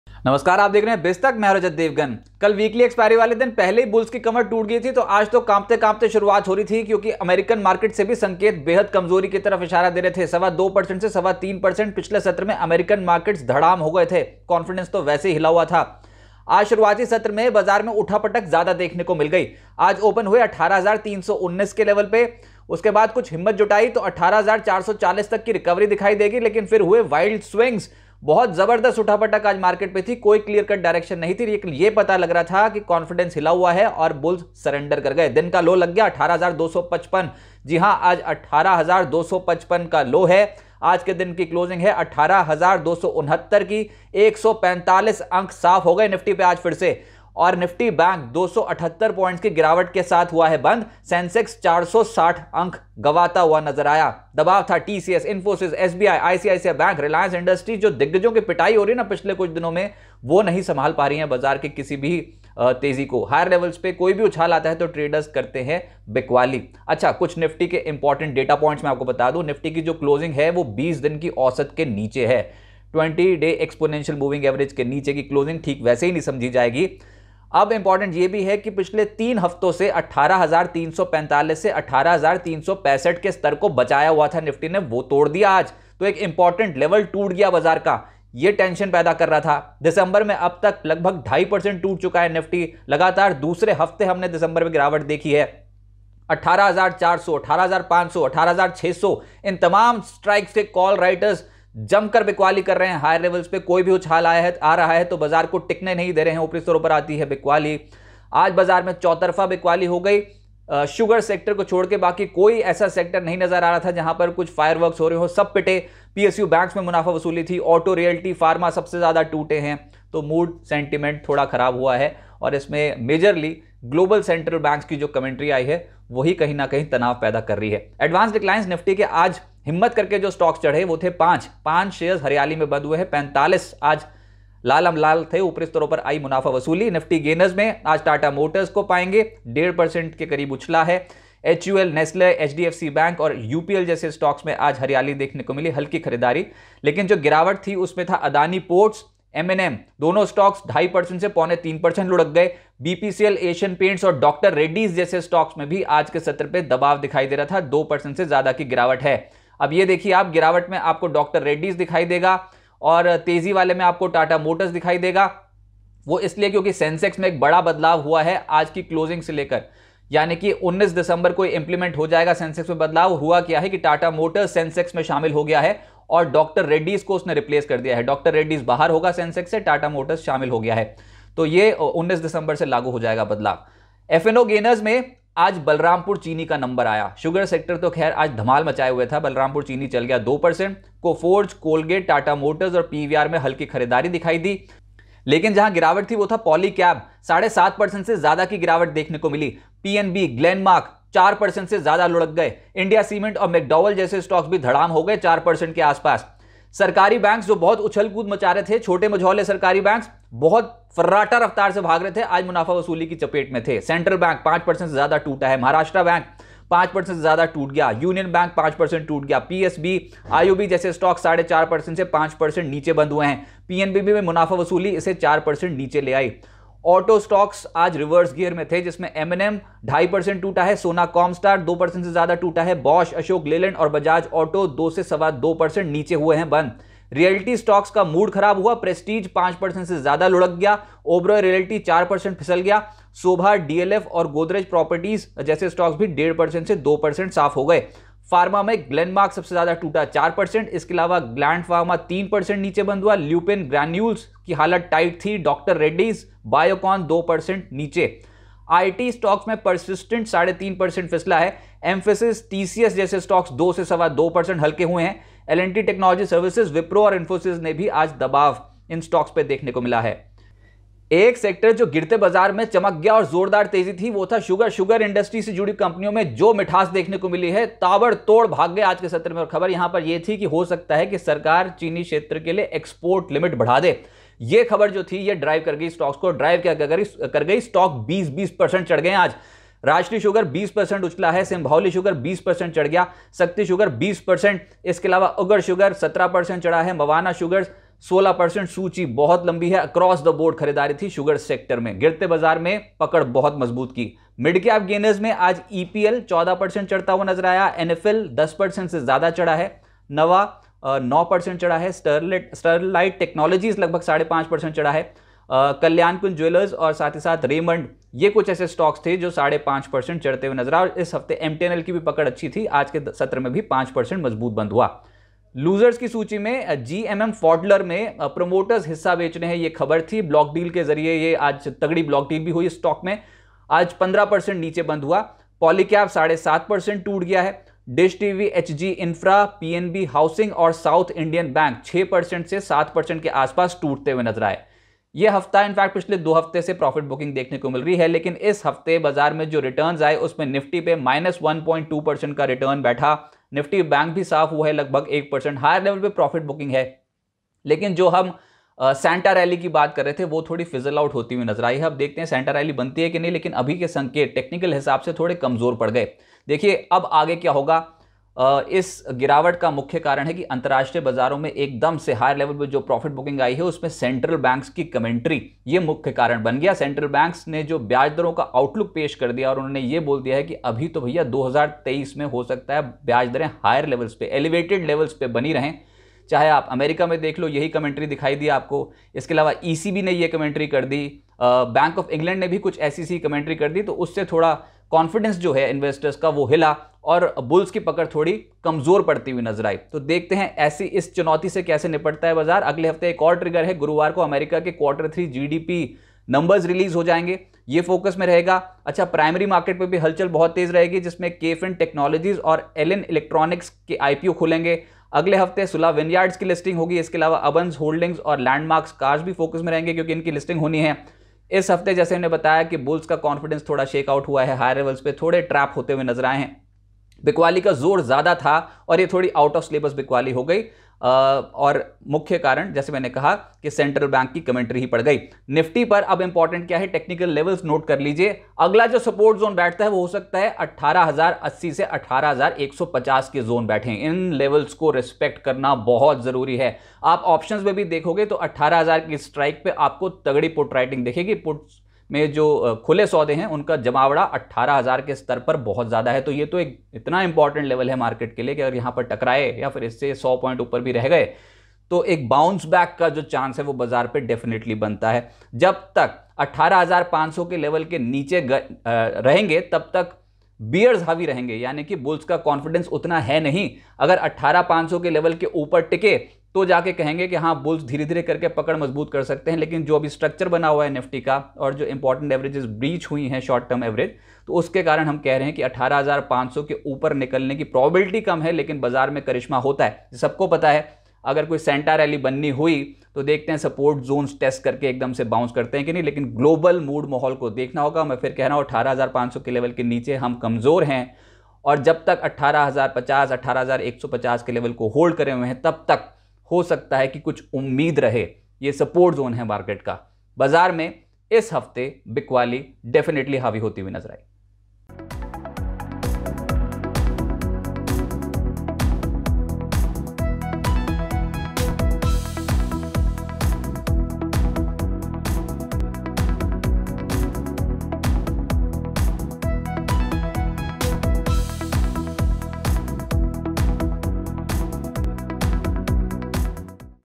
MARKET ROUNDUP | बाज़ार में टेंशन क्यों बढ़ गई? | BIZ Tak
Closing Bell: Nifty ends below 18,300, Sensex falls 461 pts; all sectors in the red